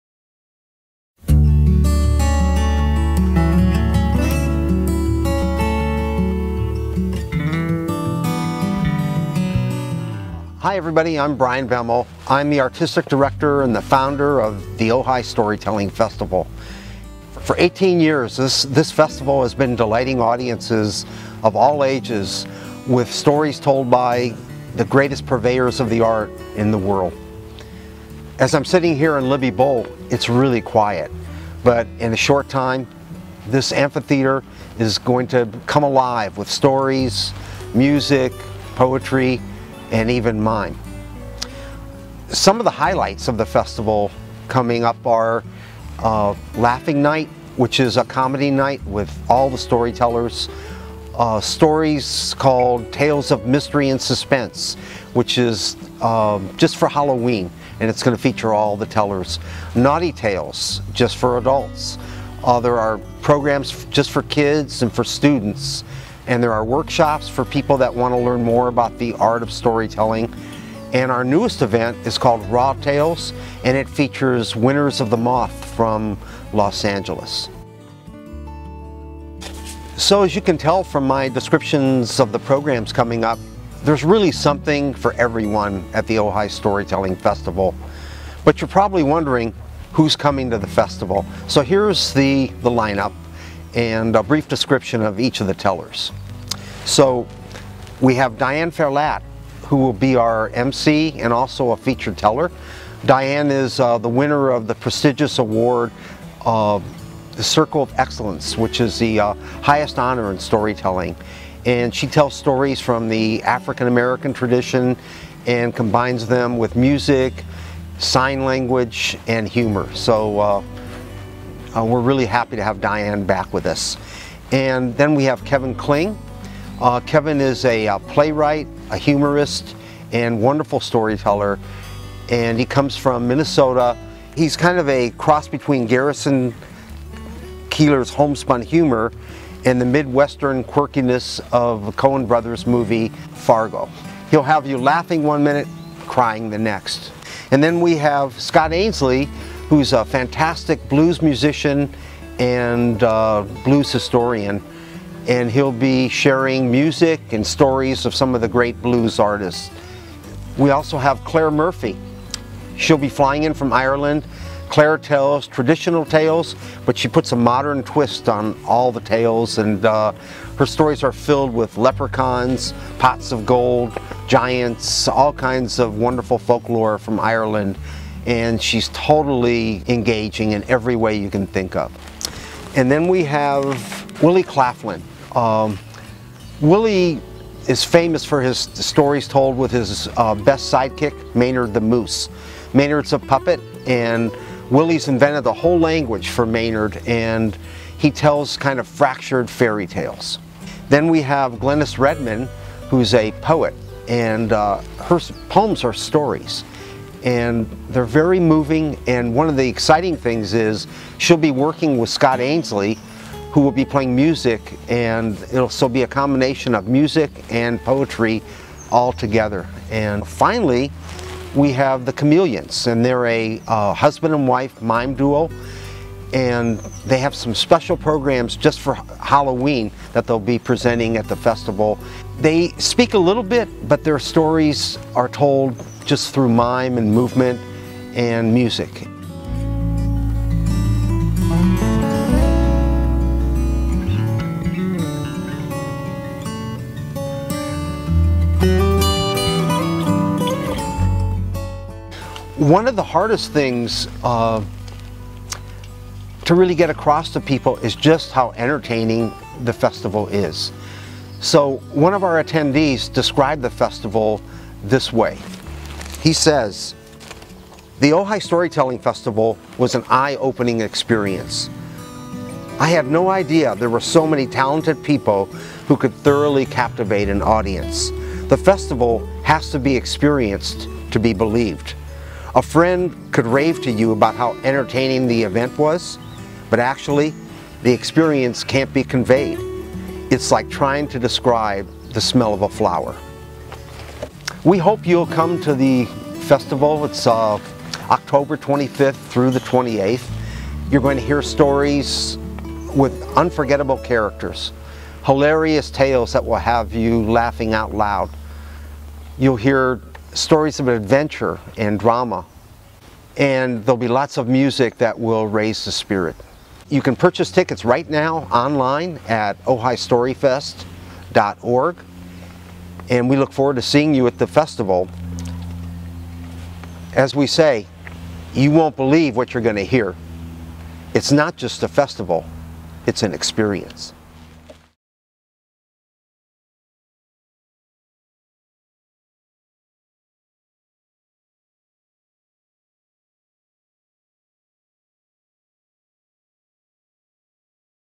Ojai Storytelling Festival 2018
The 18th Annual Ojai Storytelling Festival will feature
acclaimed storytellers:  Diane Ferlatte, Kevin Kling, Willy Claflin,
Clare Murphy, Glenis Redmond (poet extraordinaire),
Scott Ainslie (blues musician), the Chameleons (mimes),
and Moth Winners from Los Angeles.

for tickets. It's not just a festival, it's an experience.